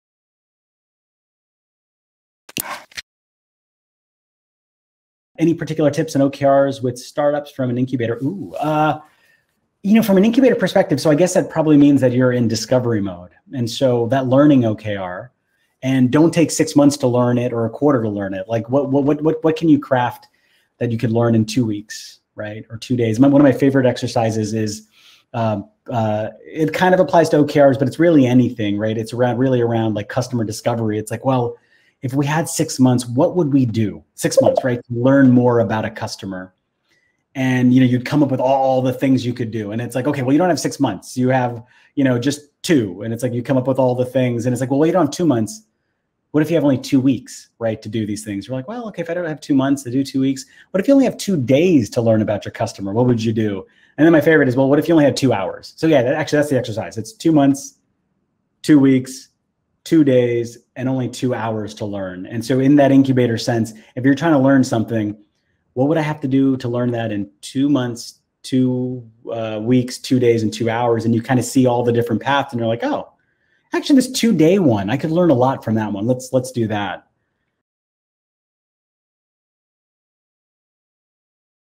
Discovery OKRs: Tips for Startups & Incubators
OKR tips for startups from an incubator perspective —is your team or organization in discovery mode? If so, how do you know what to focus on when it comes to goal setting and OKRs? In this clip, Ryan shares his favorite exercise to help startups and incubators decide what’s important as they clarify their focus with discovery OKRs. Learn more about “The Power of 2”.

Recently, Station F hosted an Ask Me Anything with Ryan Panchadsaram, Co-founder of whatmatters.com. Hosted by Roxanne Varza, Director of Station F, Ryan tackled questions from Station F entrepreneurs in Europe on his career path and Objectives and Key Results (OKRs).

⮕ For a deeper dive on using OKRs within your organization, earn your OKR Certification in our Coursera course.